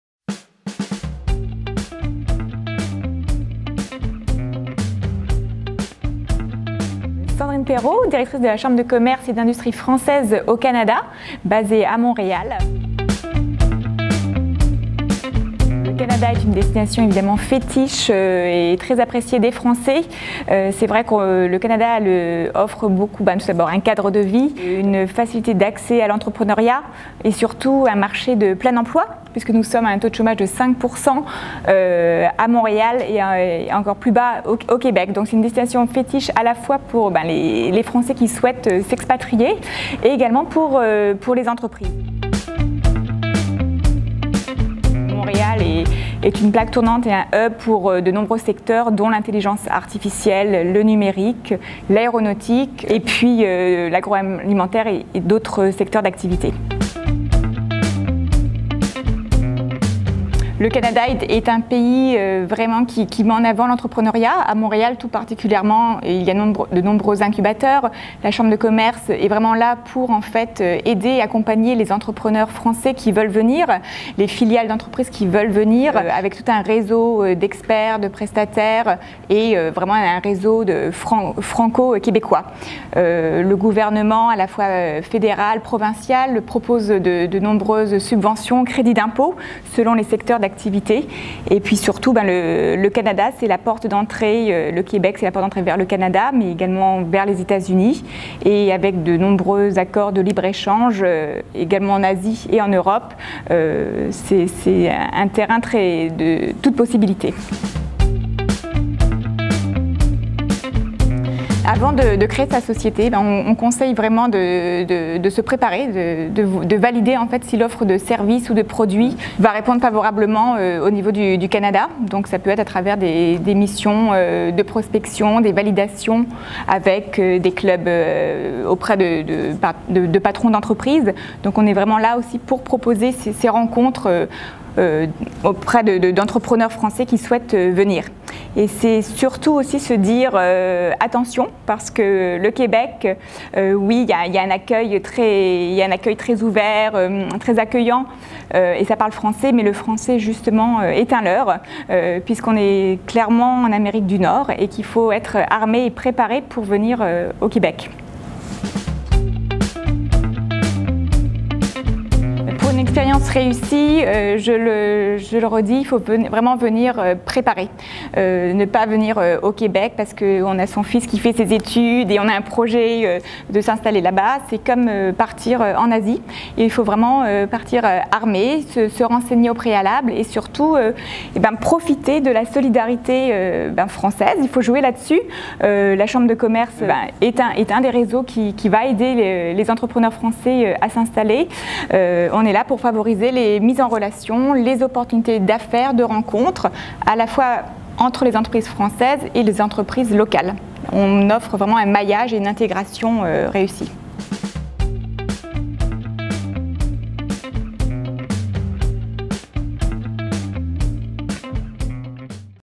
Créer une entreprise au Canada
Pourquoi entreprendre au Canada ? Cadre de vie, facilités d'accès à l'entrepreneuriat, marché de plein emploi : autant d'atouts que nous présente Sandrine Perreault, Directrice Générale de la CCI Française au Canada, basée à Montréal.

Les secteurs phares à Montréal : l'intelligence artificielle, le numérique, l'aéronautique, l'agro-alimentaire.

Réussissez votre expatriation au Canada avec expat.com !